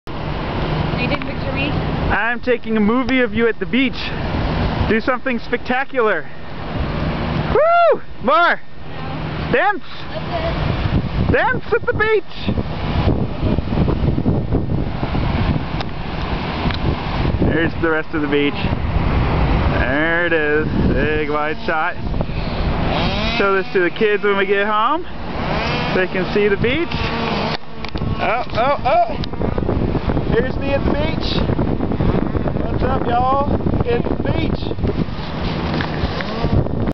What's up Y'all
My awesome girlfriend and I arrive at clearwater beach.